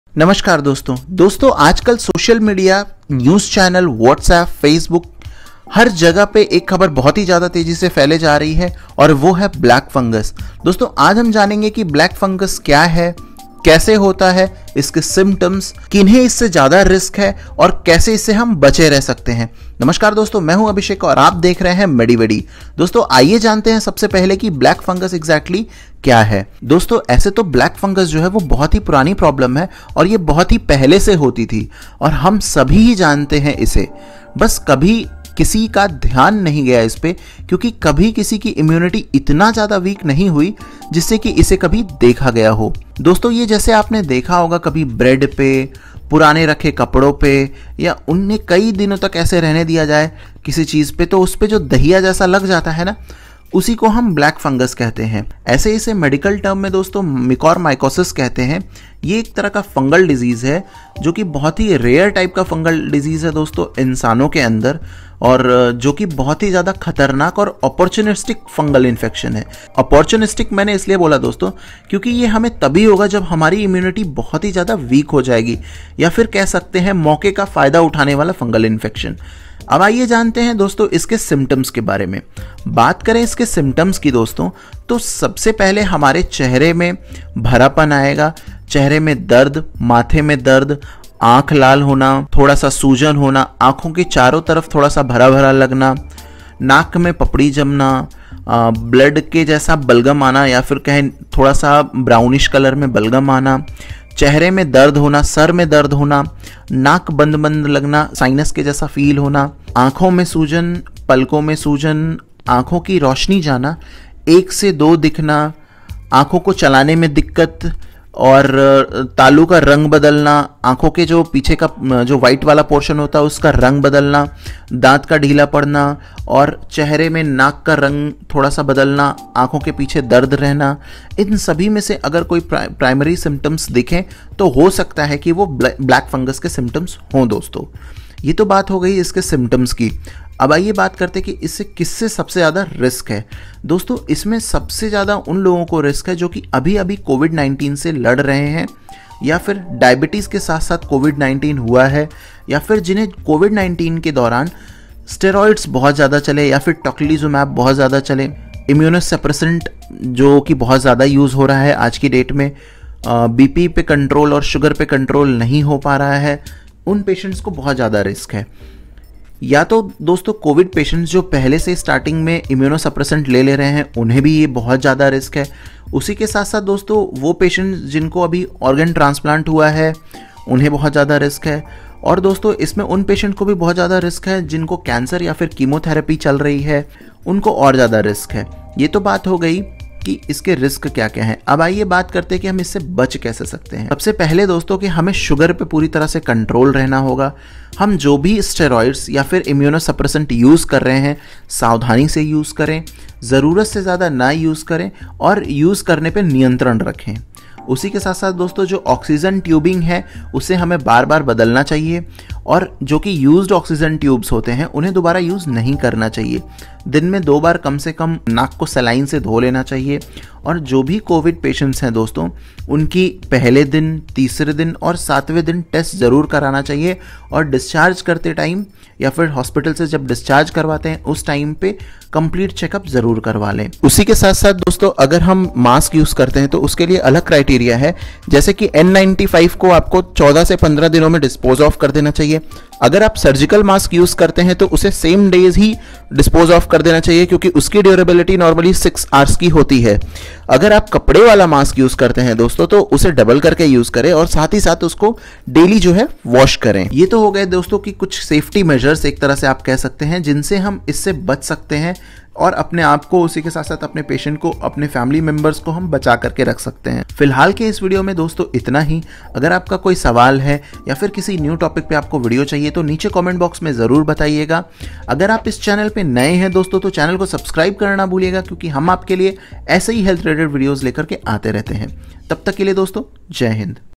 BLACK FUNGUS | ALL YOU NEED TO KNOW | PREVENTIONS | SYMPTOMS | RISK FACTORS| MediVedi
Black Fungus has become one of the most dangerous forms of fungal disease and has also been widely spreading all over the country in a very short span of time.

Recording Mic
Light stands
Light Softbox